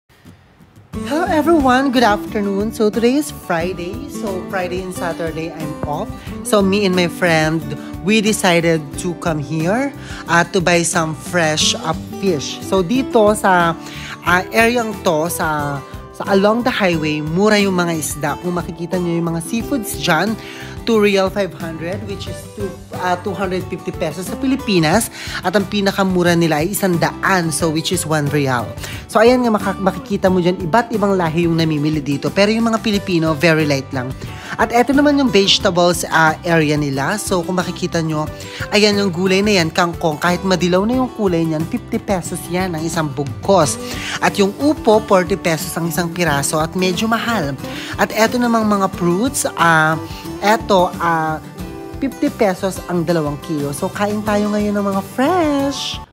Fish & Vegetable Market Along the Highway in Oman 🇴🇲
Day off natin, kaya bili tau ng fresh na isda at gulay Sa tabling daan d2 Sa Oman.